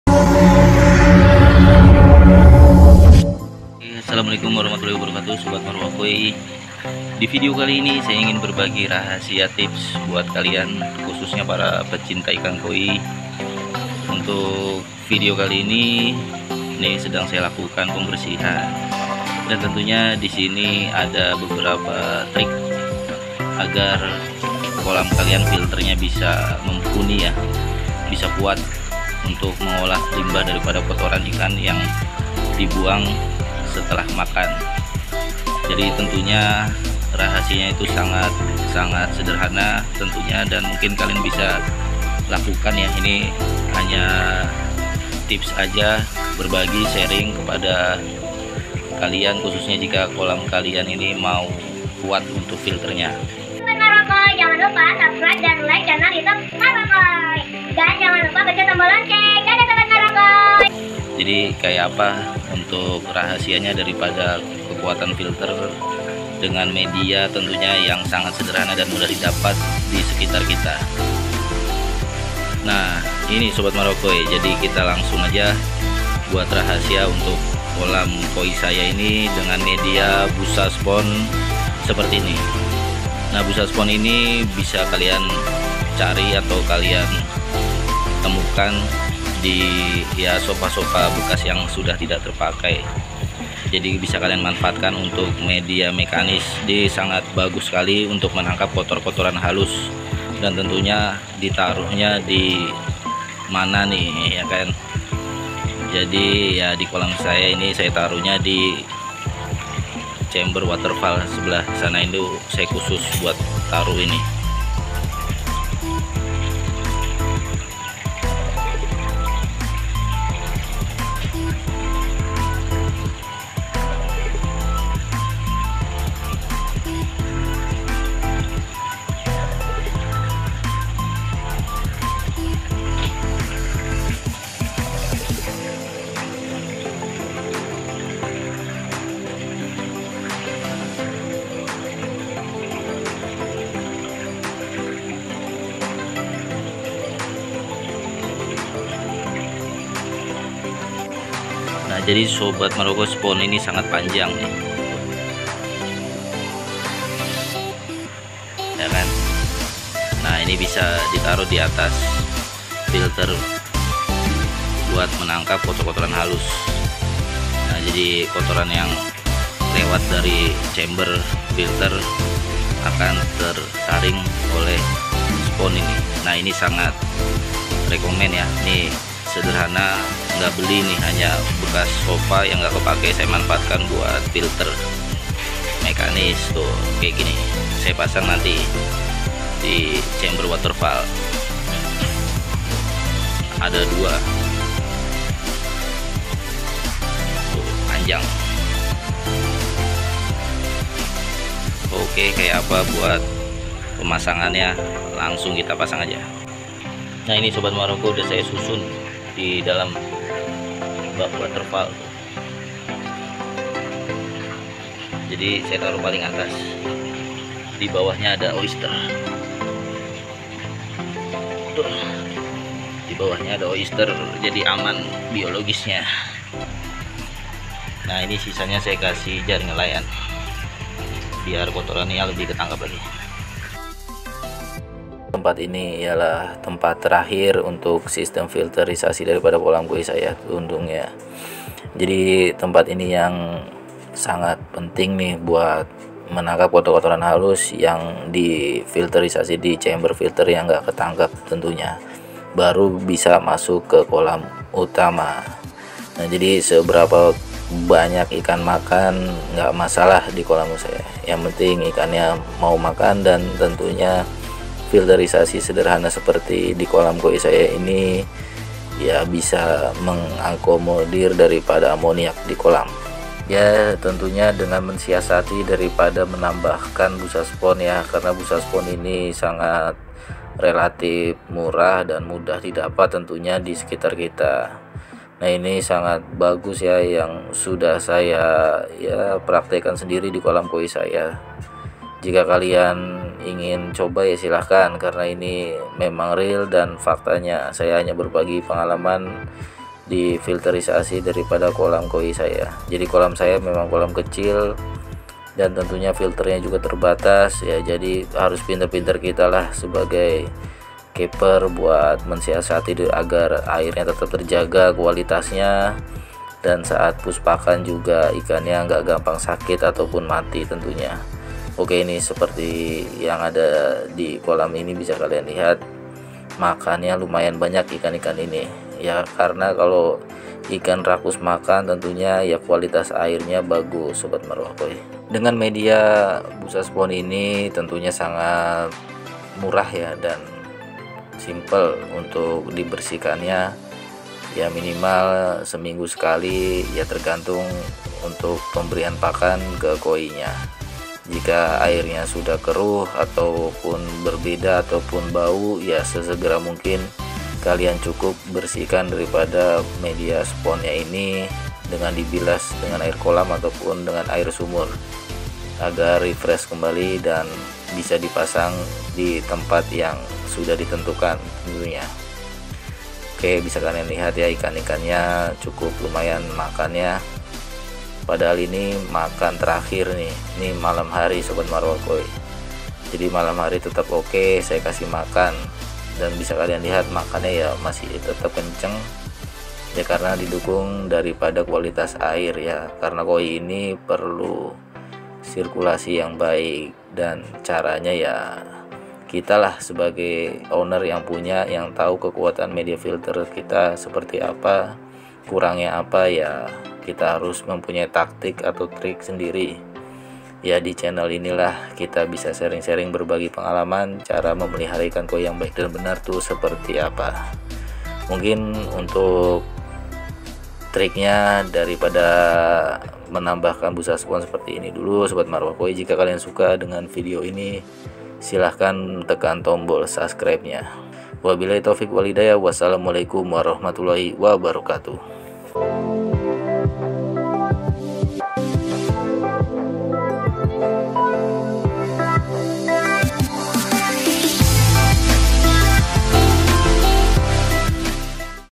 🔴 INI MANTABS⁉️ BURUAN PASANG DI KOLAM KALIAN???
MEDIA FILTER KOLAM INI SANGAT RECOMENDED UNTUK MENYARING KOTORAN HALUS YANG TIDAK BISA TERSARING DI CHAMBER...

SANGAT MUDAH DITEMUKAN DAN HASILNYA BISA KALIAN COBA...

PASANG DI CHAMBER TERAKHIR AGAR HASIL LEBIH MAKSIMAL...

MINIM MAINTANCE SOBAT